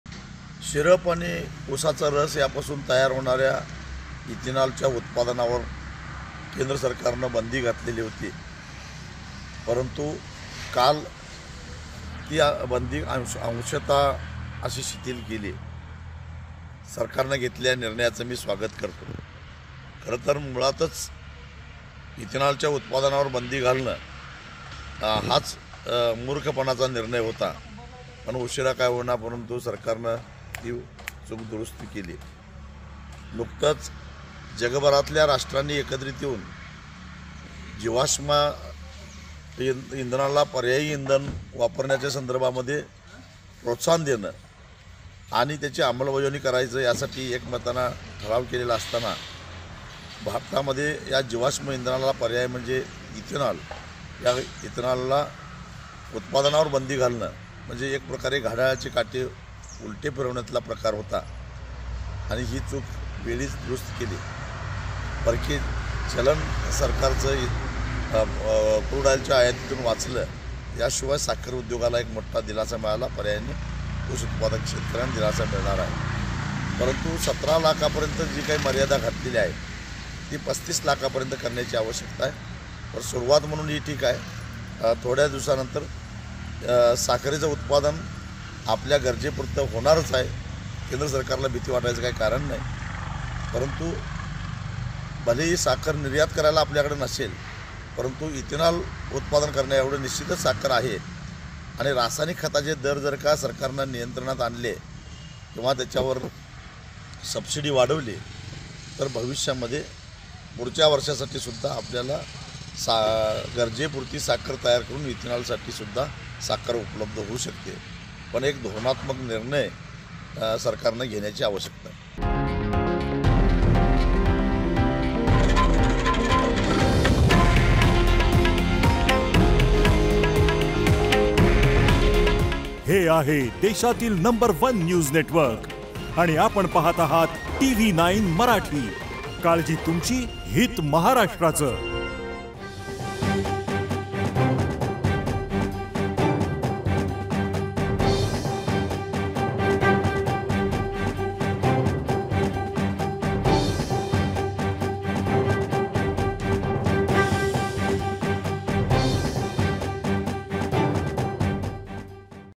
Raju Shetty On Gove | सरकारच्या इथेनॉल निर्मिती बंदीच्या निर्णयावर राजू शेट्टी यांची प्रतिक्रिया
CREDIT - Political| Amar Bhingardeve /Producer| Shruti Gosavi /Editor